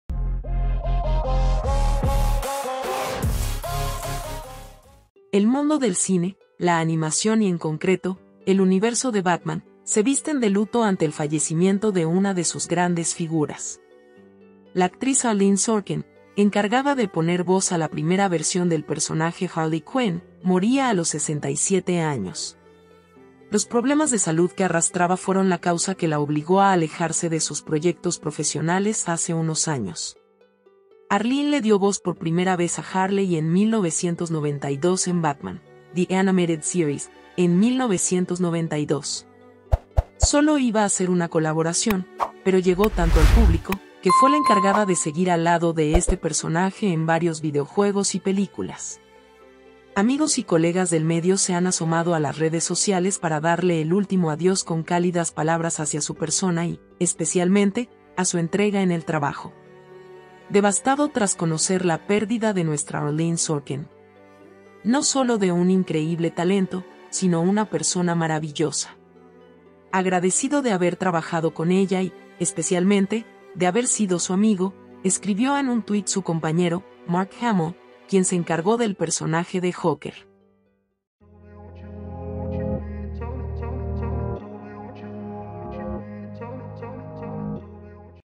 Muere la actriz Arleen Sorkin, la voz original de Harley Quinn en Batman 💔🕊
La actriz Arleen Sorkin, encargada de poner voz a la primera versión del personaje Harley Quinn, moría a los 67 años.

Los problemas de salud que arrastraba fueron la causa que la obligó a alejarse de sus proyectos profesionales hace unos años.